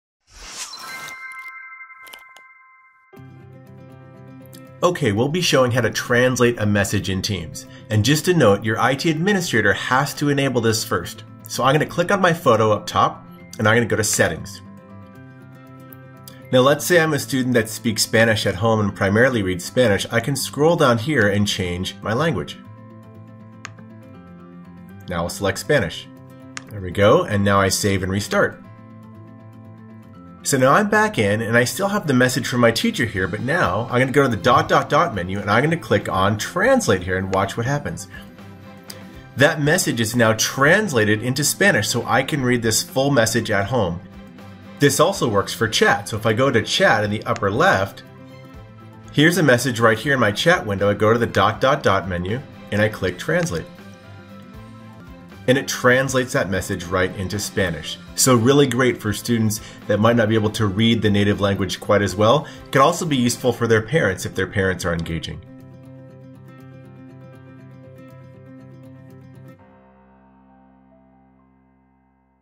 How to translate messages in Microsoft Teams!
Learn how to easily translate a message post or chat message into another language with Microsoft Teams. Help ensure full inclusion of all student in your class.  Over 60 languages supported!